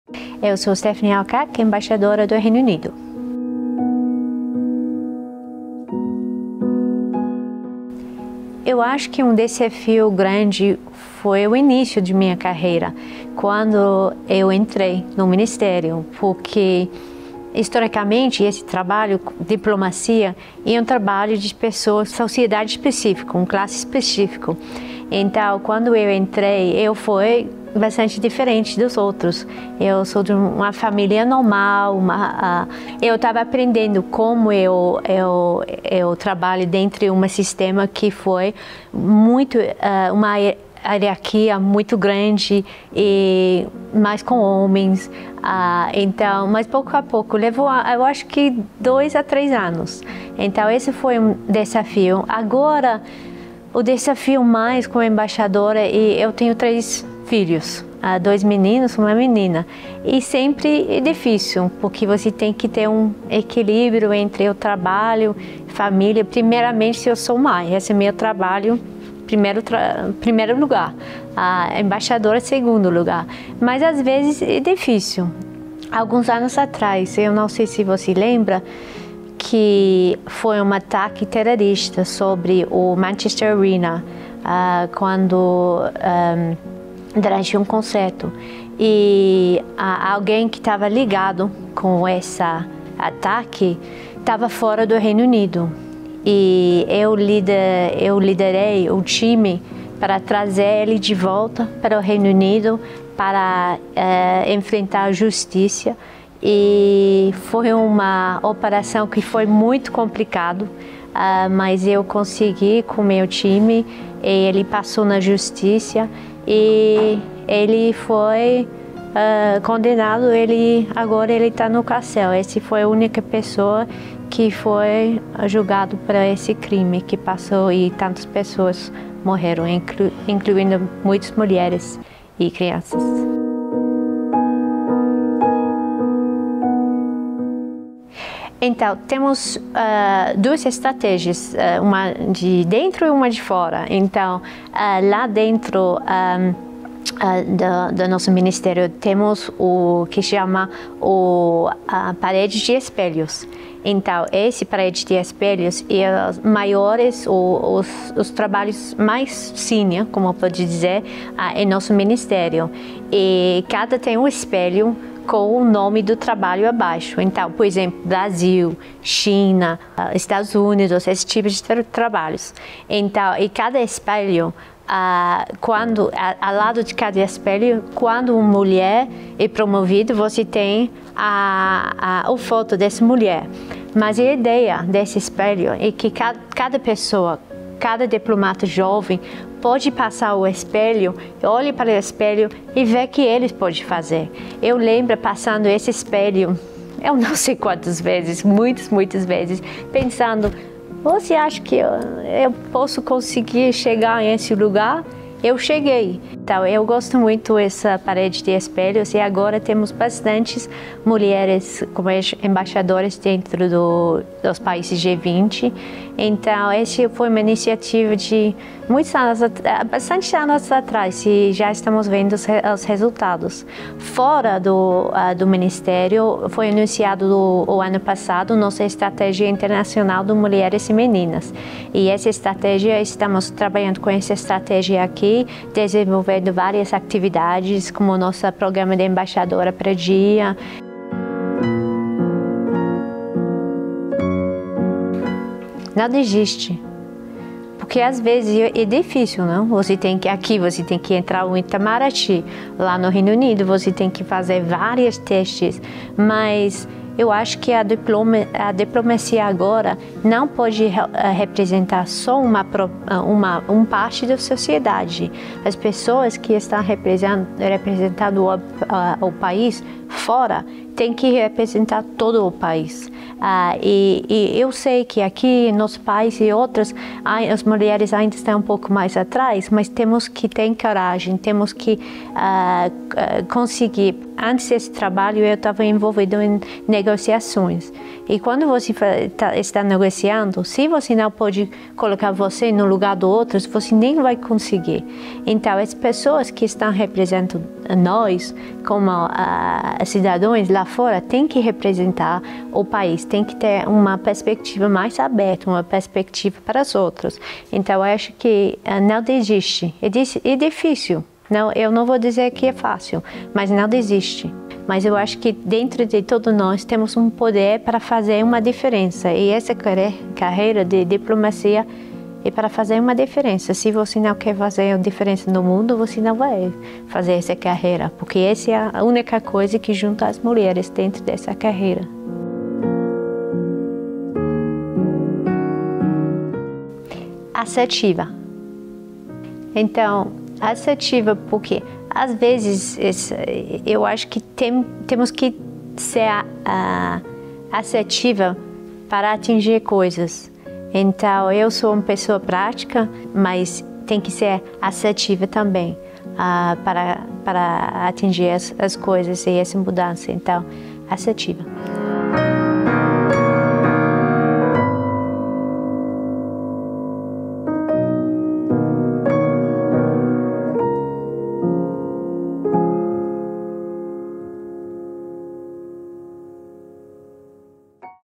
CasaMundo - Embaixadora do Reino Unido - Stephanie Al Qaq
O episódio de hoje do Mulheres na Diplomacia, projeto GPS em colaboração com Renata Zuquim, apresenta a embaixadora do Reino Unido no Brasil, Stephanie Al-Qaq.

Em seu depoimento, ela evidencia a importância da representatividade na diplomacia, haja vista a relevância da alteridade nas negociações internacionais.

A embaixadora também relata a sua experiência com a Parede de Espelhos, instalada em seu Ministério para incentivar a ascensão de mulheres na carreira diplomática.